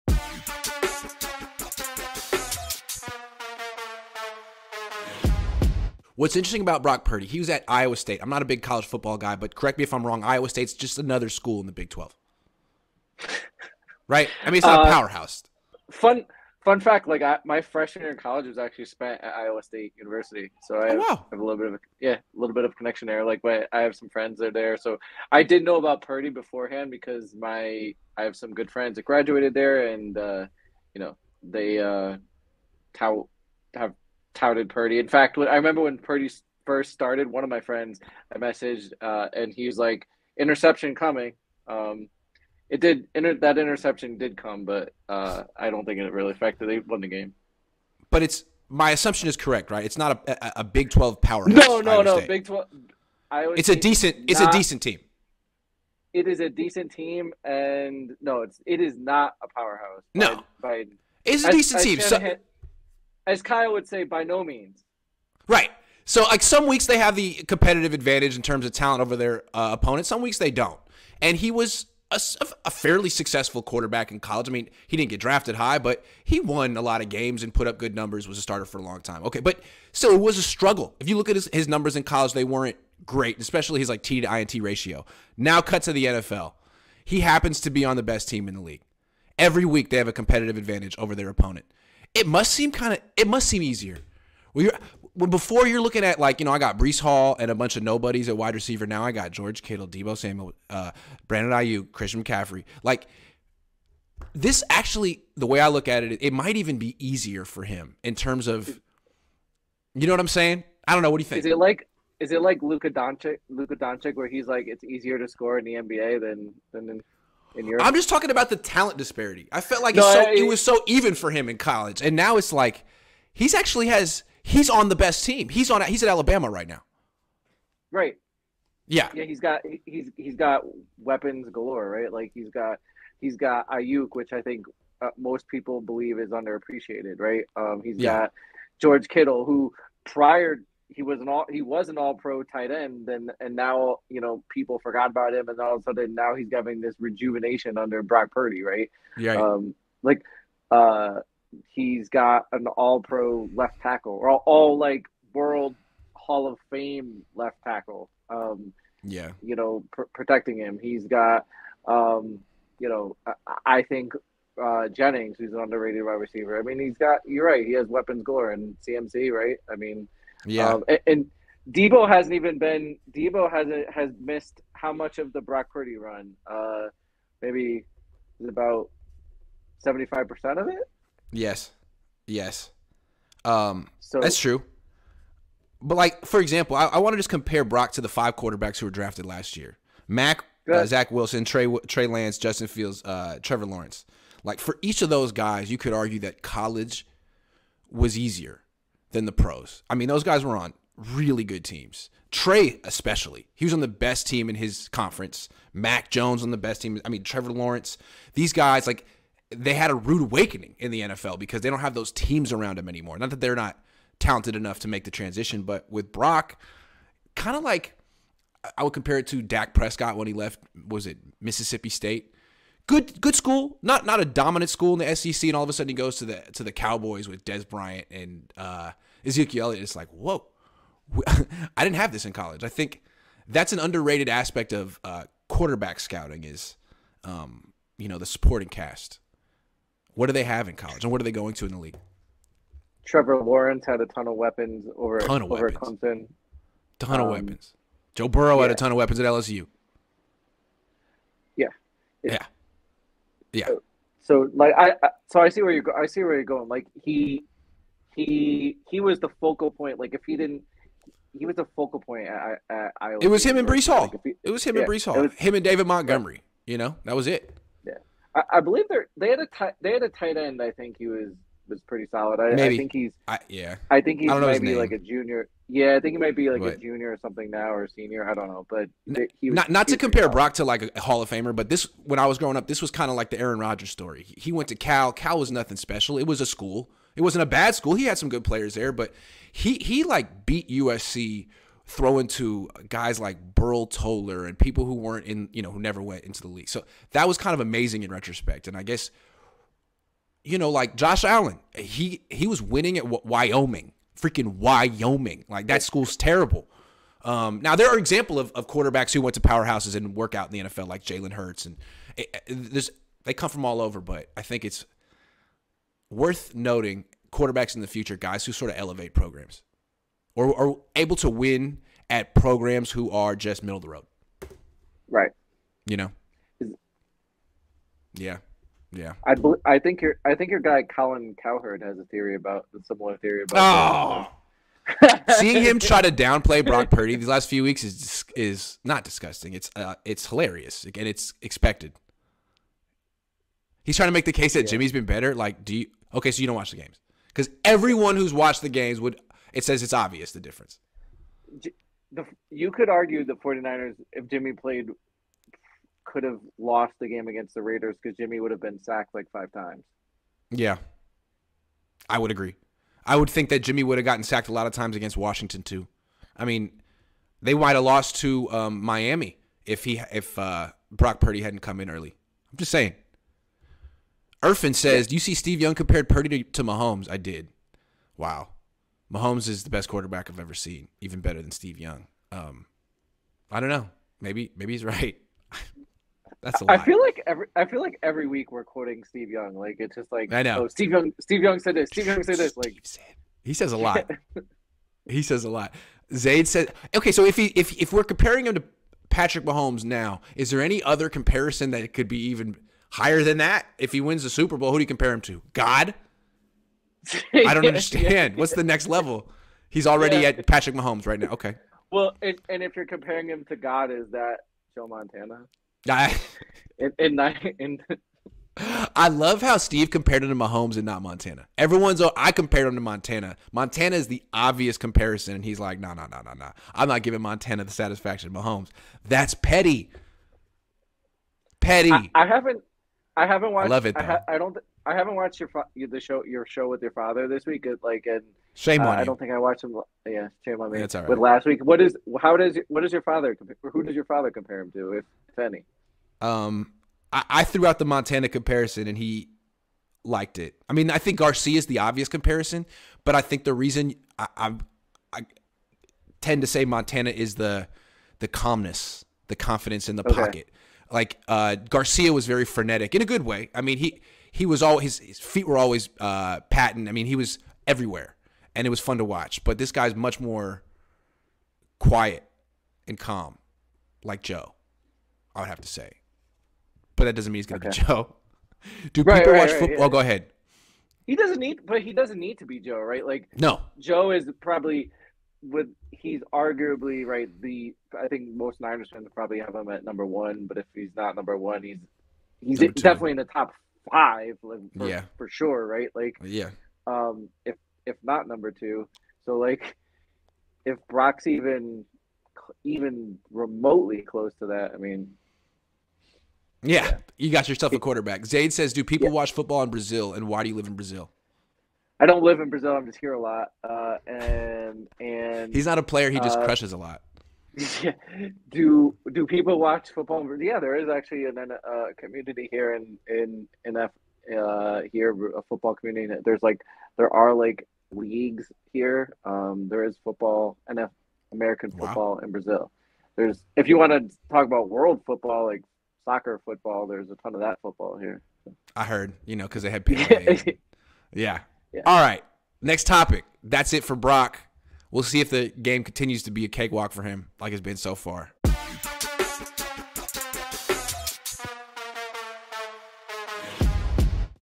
Why 49ers QB Brock Purdy’s Transition from College to the NFL Has Been So Easy
Grant Cohn and Coach Noah Johnston discuss why San Francisco 49ers quarterback Brock Purdy’s transition from college to to the NFL has been so easy.